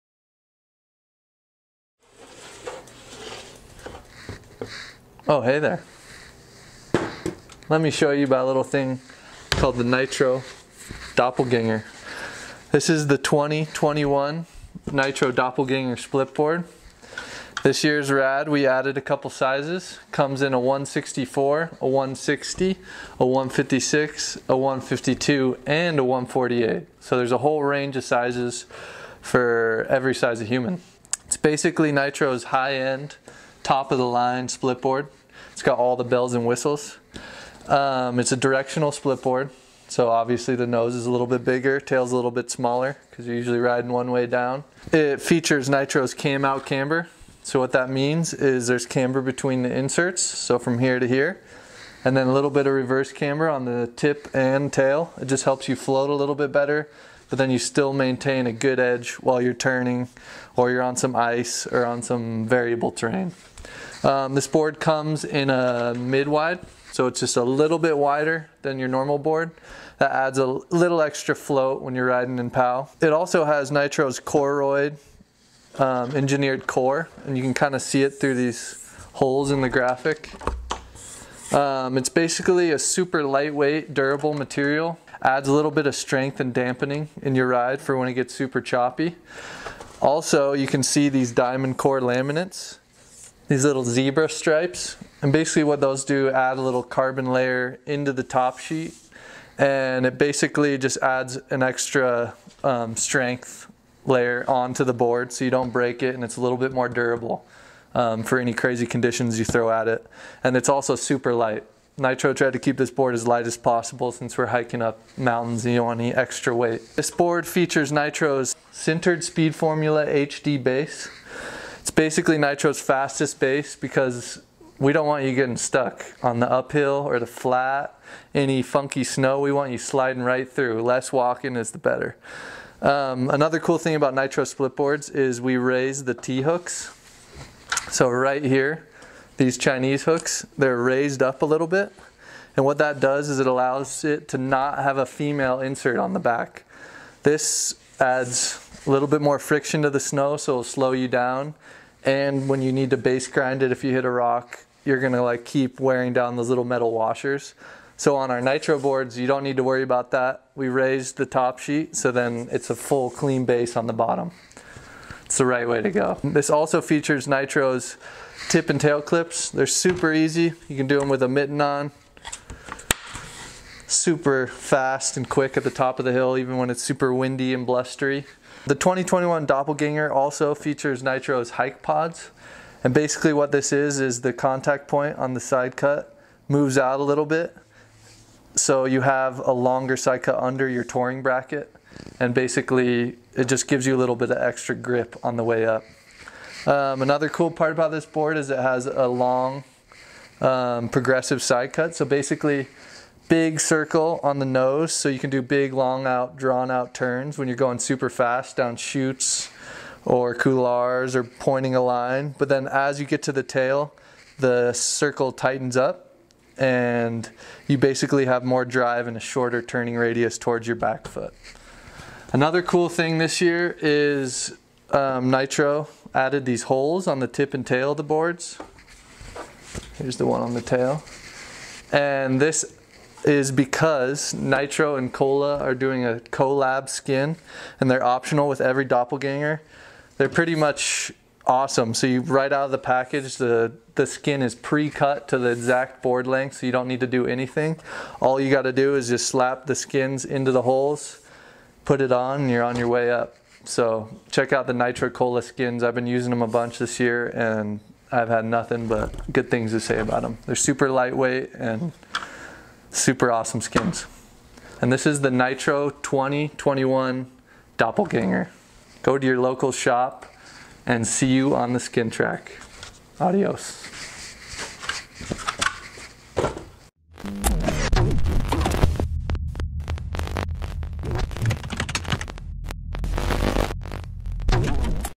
21-22 NITRO ナイトロ スノーボード -DOPPELEGANGER(ドッペルゲンガー）-（日本語字幕入り）
ナイトロDOPPLEGANGER（ドッペルゲンガー）は、Koroyd®パワーコアを使用した超軽量スプリットボード。劇的な軽量化に成功し体力を温存できるだけでなく、スキンの装着も楽にでき、未開のパウダーゾーンへの探求をサポート。